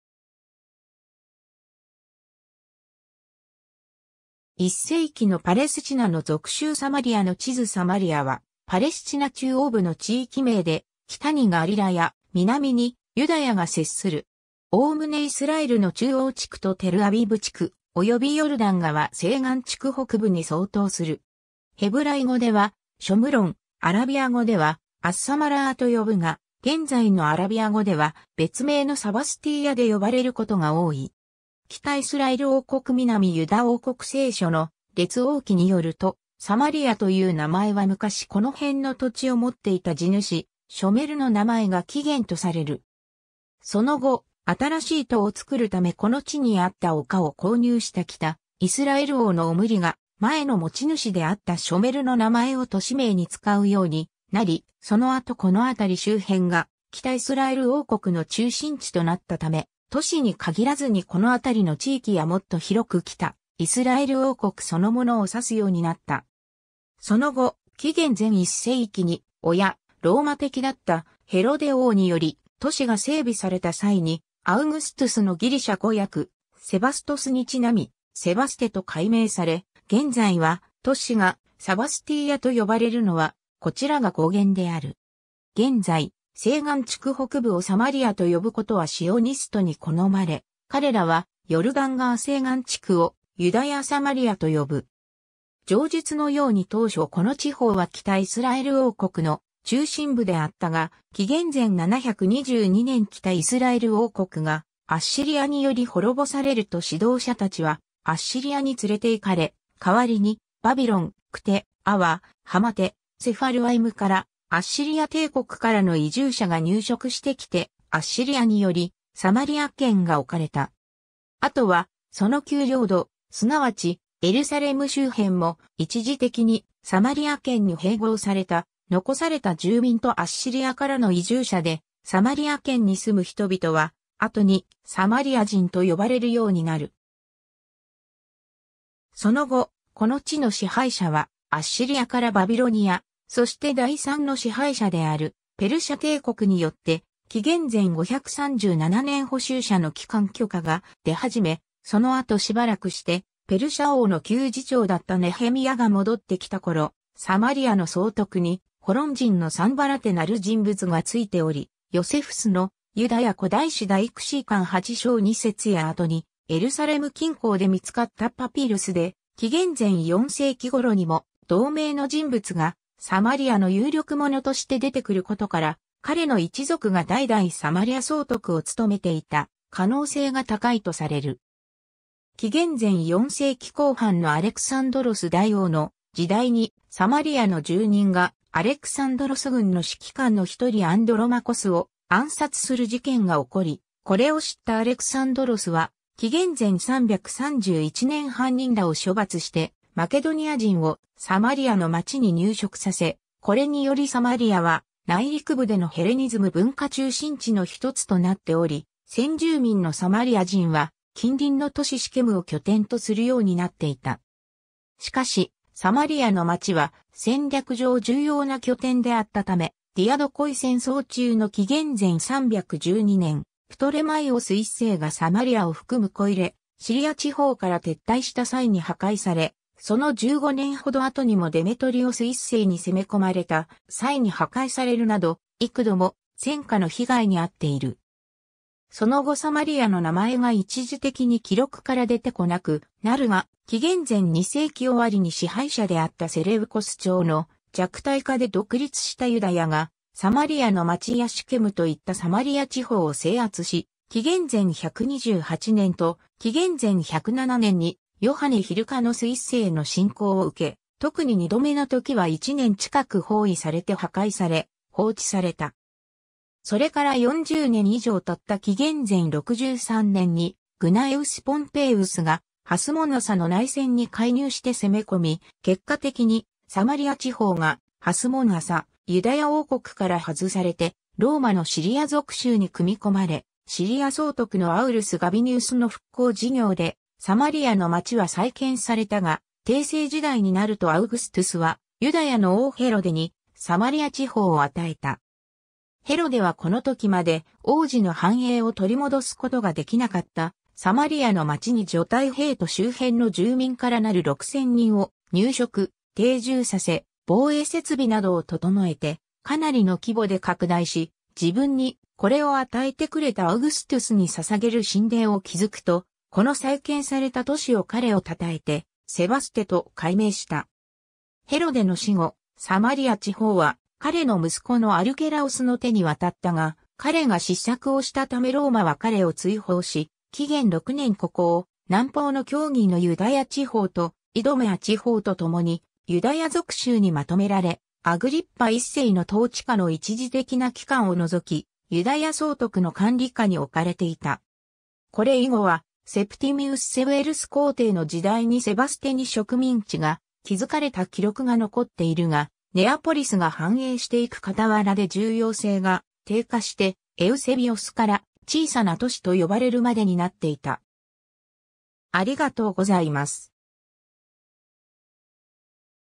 サマリア
1世紀のパレスチナの属州 サマリアの地図（1894年） サマリア（Samaria）は、パレスチナ中央部の地域名で、北にガリラヤ、南にユダヤが接する。
おおむねイスラエルの中央地区とテルアビブ地区、およびヨルダン川西岸地区北部に相当する。
 ヘブライ語ではショムロン（שומרון、Shomron）、アラビア語ではアッサマラー（السامرة‎、as-Sāmarah）と呼ぶが、現在のアラビア語では別名のサバスティーヤ(Sabastīya)で呼ばれることが多い。
 北イスラエル王国 南ユダ王国 聖書の『列王記』によると、サマリアという名前は昔この辺の土地を持っていた地主「ショメル（Shemer　セメルとも）」の名前が起源とされる（日本語読みに直すと分かりにくいが、原語では「ショメル」「ショムロン」で似た音であることが分かる）。
その後、新しい都を作るためこの地にあった丘を購入した北イスラエル王のオムリが前の持ち主であったショメルの名前を都市名に使うようになり、その後この辺り周辺が北イスラエル王国の中心地となったため、都市に限らずにこのあたりの地域やもっと広く北イスラエル王国そのものを指すようになった。
 その後、紀元前1世紀に親ローマ的だったヘロデ王により都市が整備された際に、アウグストゥスのギリシャ語訳セバストスに因み「セバステ (Σεβαστη　尊敬すべし) 」と改名され、現在は都市が「サバスティーヤ」と呼ばれるのはこちらが語源である。
 現在、西岸地区北部をサマリアと呼ぶことはシオニストに好まれ、彼らはヨルダン川西岸地区をユダヤ・サマリアと呼ぶ。
 上述のように当初この地方は北イスラエル王国の中心部であったが、紀元前722年北イスラエル王国がアッシリアにより滅ぼされると指導者たちはアッシリアに連れていかれ（アッシリア捕囚）、代わりにバビロン、クテ、アワ、ハマテ、セファルワイムからアッシリア帝国からの移住者が入植してきて、アッシリアによりサマリア県が置かれた。
 （サマリア県はアッシリアに続くバビロニア時代にも存続し、紀元前586年の南ユダ王国滅亡（バビロン捕囚）後は、その旧領土、すなわちエルサレム周辺も、一時的にサマリア県に併合された） 残された住民とアッシリアからの移住者でサマリア県（狭義）に住む人々は、後に「サマリア人」と呼ばれるようになる。
 その後、この地の支配者はアッシリアからバビロニア、そして第3の支配者であるペルシャ帝国によって紀元前537年捕囚者の帰還許可が出始め、その後しばらくしてペルシャ王の給仕長だったネヘミヤが戻ってきた頃、サマリアの総督に「ホロン人のサンバラテ（サンバラト）」なる人物がついており、ヨセフスの『ユダヤ古代誌』第XI巻8章2節や後にエルサレム近郊で見つかったパピルスで紀元前4世紀頃にも同名の人物がサマリアの有力者として出てくることから、彼の一族が代々サマリア総督を務めていた可能性が高いとされる。
 紀元前4世紀後半のアレクサンドロス大王の時代にサマリアの住人がアレクサンドロス軍の指揮官の一人アンドロマコスを暗殺する事件が起こり、これを知ったアレクサンドロスは紀元前331年犯人らを処罰してマケドニア人をサマリアの街に入植させ、これによりサマリアは内陸部でのヘレニズム文化中心地のひとつとなっており、先住民のサマリア人は近隣の都市シケムを拠点とするようになっていた。
 しかし、サマリアの街は戦略上重要な拠点であったためディアドコイ戦争中の紀元前312年、プトレマイオス1世がサマリアを含むコイレ・シリア地方から撤退した際に破壊され、その15年ほど後にもデメトリオス1世に攻め込まれた際に破壊されるなど、幾度も戦火の被害に遭っている（破壊されたのかは不明だが、第4次・第5次シリア戦争の両方でもアンティオコス3世にサマリアが占領されたことがある）。
 その後サマリアの名前が一時的に記録から出てこなくなるが、紀元前2世紀終わりに支配者であったセレウコス朝の弱体化で独立したユダヤ（ハスモン朝）がサマリアの街やシケムといったサマリア地方を制圧し、紀元前128年と紀元前107年にヨハネ・ヒルカノス1世の侵攻を受け、特に二度目の時は1年近く包囲されて破壊され放置された。
 それから40年以上たった紀元前63年にグナエウス・ポンペイウスがハスモン朝の内戦に介入して攻め込み、結果的にサマリア地方がハスモン朝ユダヤ王国から外されてローマのシリア属州に組み込まれ、シリア総督のアウルス・ガビニウスの復興事業...